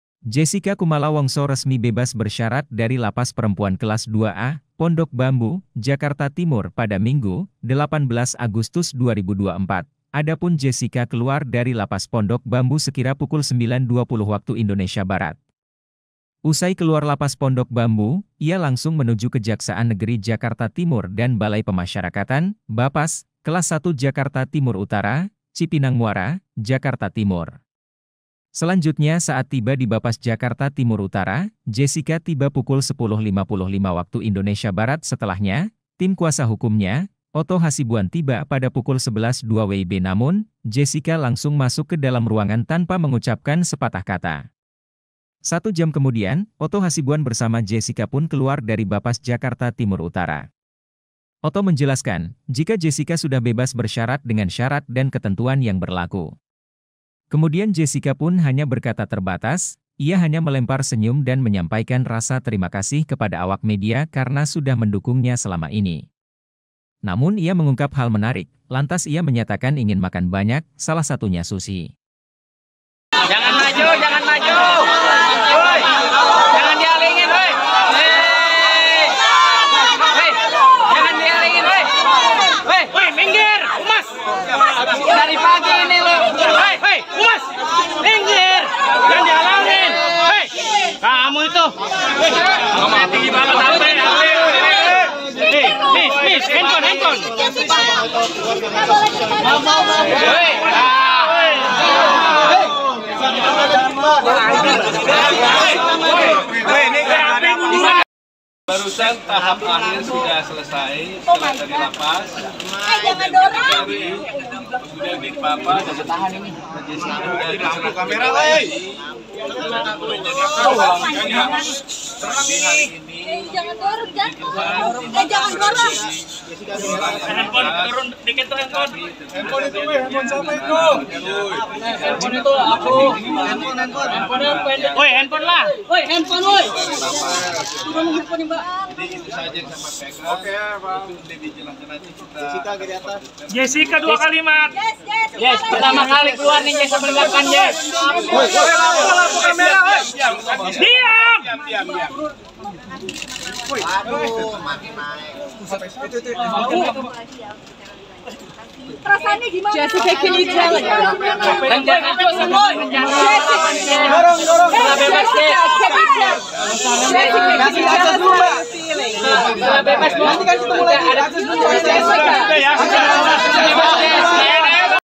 Jessica Wongso Bebas Bersyarat dari Lapas Pondok Bambu, Rindu Makan Banyak Salah Satunya Sushi
WARTAKOTALIVE.COM, JAKARTA - Jessica Kumala Wongso resmi bebas bersyarat dari Lapas Perempuan Kelas II A, Pondok Bambu, Jakarta Timur pada Minggu (18/8/2024)

Adapun Jessica keluar dari Lapas Pondok Bambu sekira pukul 09.20 WIB.

Usai keluar Lapas Pondok Bambu, ia langsung menuju Kejaksaan Negeri Jakarta Timur dan Balai Pemasyarakatan (Bapas) Kelas I Jakarta Timur-Utara, Cipinang Muara, Jakarta Timur.